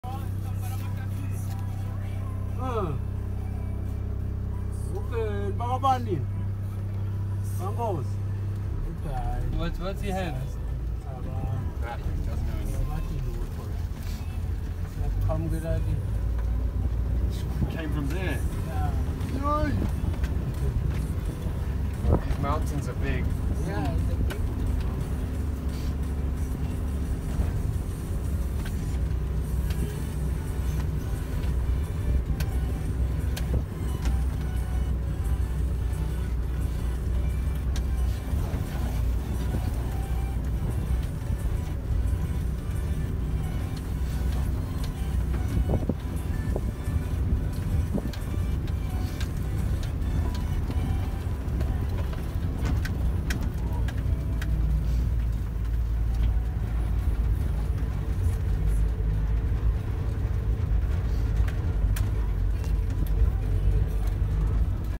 Taking the Swaziland Army to fight Zuma Zuma illegal miners in eSwatini for gold.
Bitcoin: 37MhZLTyra9DpXAKKbuDtfax2baTNHNuX6

ETH:
0x5e25f9b6e81746ee9be3760ecd7967e6080ad673

Permaculture and Organic Garden Build out in eSwatini